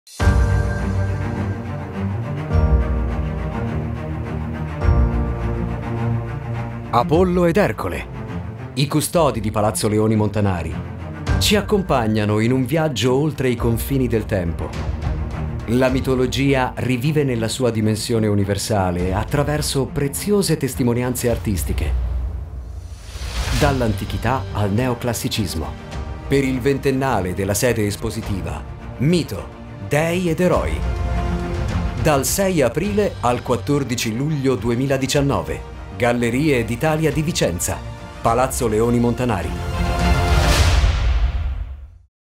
Mostra "Mito. Dei ed Eroi" | Gallerie d'Italia di Vicenza
Il 2019 è un anno importante per Palazzo Leoni Montanari: la nostra sede di Vicenza compie vent'anni e per festeggiare in grande abbiamo organizzato la mostra "Mito. Dei ed Eroi", un viaggio nella fortuna e nell'esemplarità dei miti classici partendo dalla sua rappresentazione nell’antichità per arrivare all’età neoclassica.

"Mito. Dei ed Eroi”
6 aprile - 14 luglio 2019
GalleriedItalia di Vicenza

Seguici sul sito e sui social di Gallerie d’Italia per essere sempre aggiornato sulle mostre e sulle iniziative in corso: